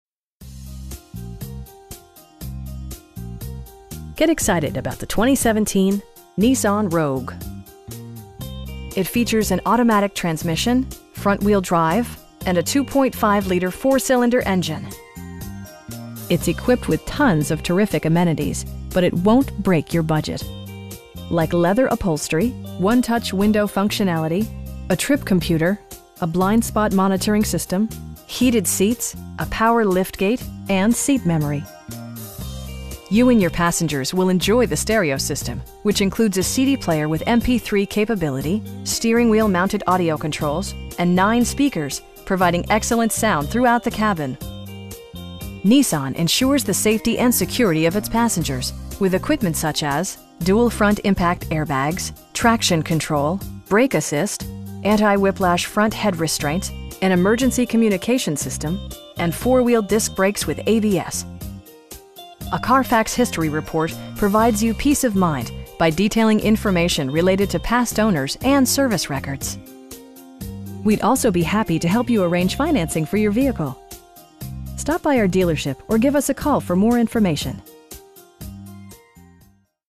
2017 Nissan Rogue SL in Austin, TX 78758
Get excited about the 2017 Nissan Rogue. With less than 20,000 miles on the odometer, this 4 door sport utility vehicle prioritizes comfort, safety and convenience. It features an automatic transmission, front-wheel drive, and a 2.5 liter 4 cylinder engine. It's equipped with tons of terrific amenities, but it won't break your budget. Like leather upholstery, 1-touch window functionality, a trip computer, a blind spot monitoring system, heated seats, a power liftgate, power windows, and seat memory. You and your passengers will enjoy the stereo system, which includes a CD player with MP3 capability, steering wheel mounted audio controls, and 9 speakers, providing excellent sound throughout the cabin. Nissan ensures the safety and security of its passengers with equipment such as: dual front impact airbags, head curtain airbags, traction control, brake assist, anti-whiplash front head restraints, a security system, an emergency communication system, and 4 wheel disc brakes with ABS. For added security, dynamic Stability Control supplements the drivetrain. A Carfax history report provides you peace of mind by detailing information related to past owners and service records. We'd also be happy to help you arrange financing for your vehicle. Stop by our dealership or give us a call for more information.